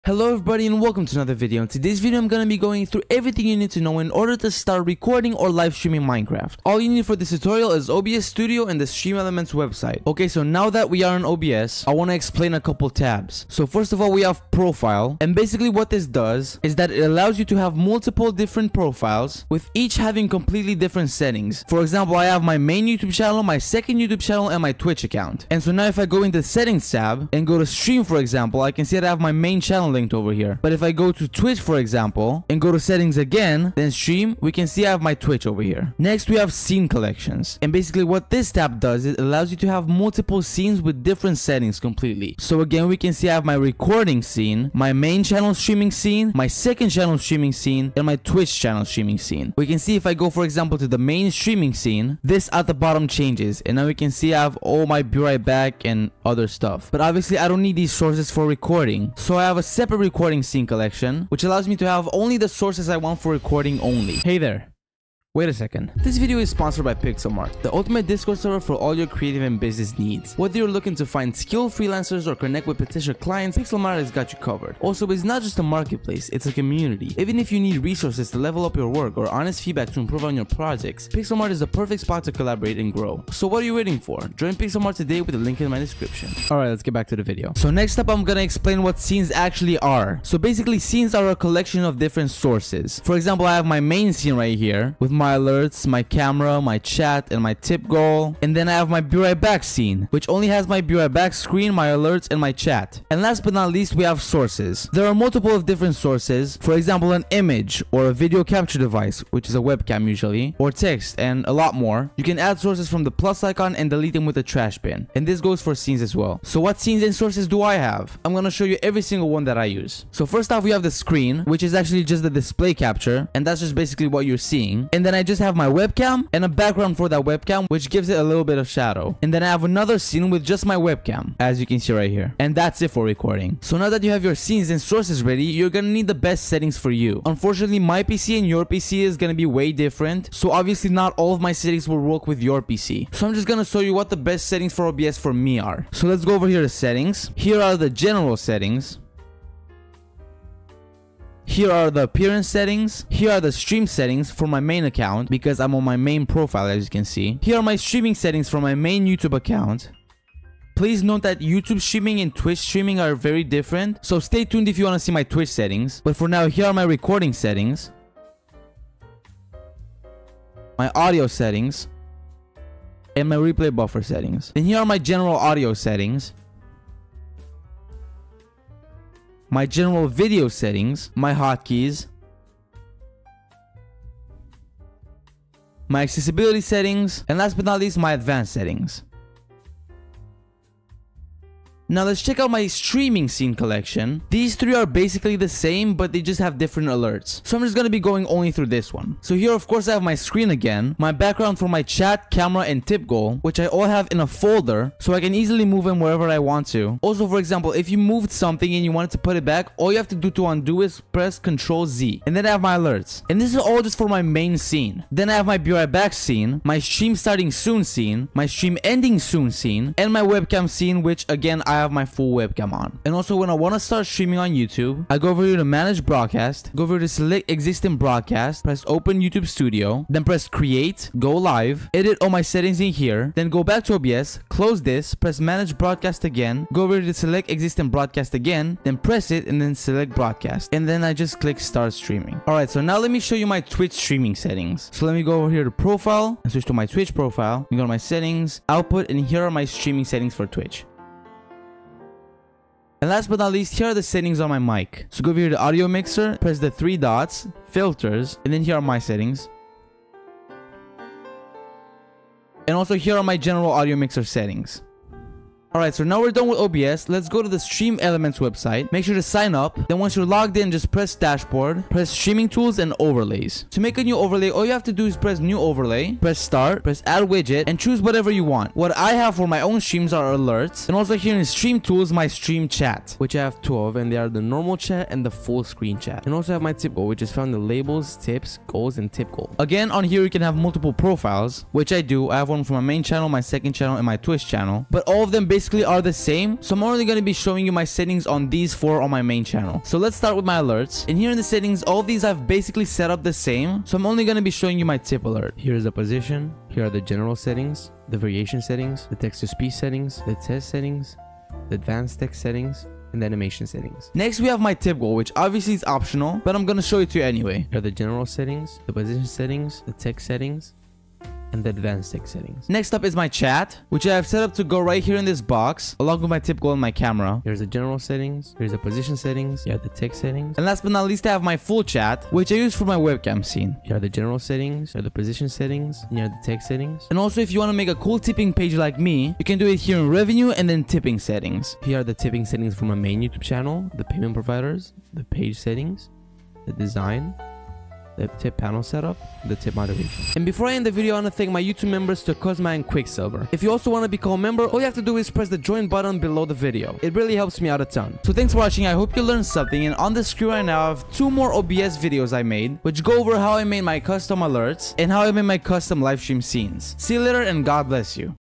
How To Record & Livestream Minecraft
ign: Pqtric

ToCosma, Quiksilver

Chapters:
00:00 - Intro
00:10 - Profile
00:36 - Scene Collection
01:27 - Scenes
01:44 - Sources
02:17 - Settings + YouTube Streaming
03:12 - Streaming Scene Collection
03:43 - Manage Broadcast
04:04 - Twitch Streaming
04:14 - Audio Settings
04:26 - Overlays
05:00 - Overlay Settings
05:37 - Tipping Page Settings